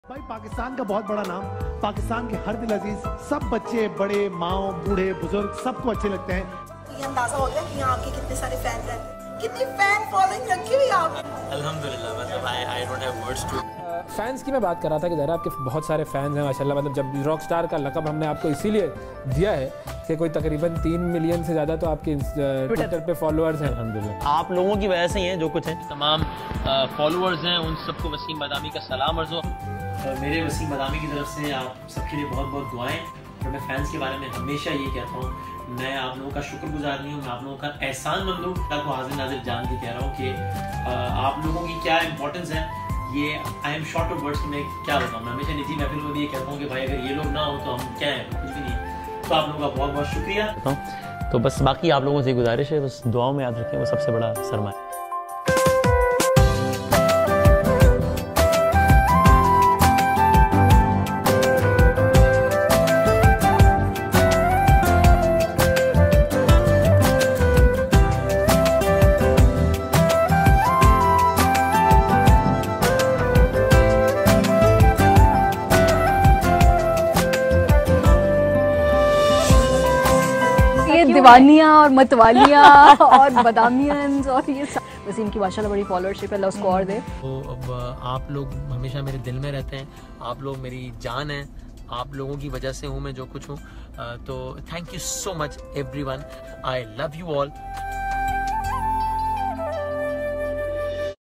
Followers - A family ~ WaseemBadami x Social Media Fans
Followers ~ A Family❤️
|@WaseemBadami x SocialMedia Fans|

Congrats on 2M on Insta & 3.8M Followers on Twitter WB..Proud to be a fan of someone as passionate & genuine as u! I feel accomplished having u as my guide.
Thankyou for teaching me towards the better💕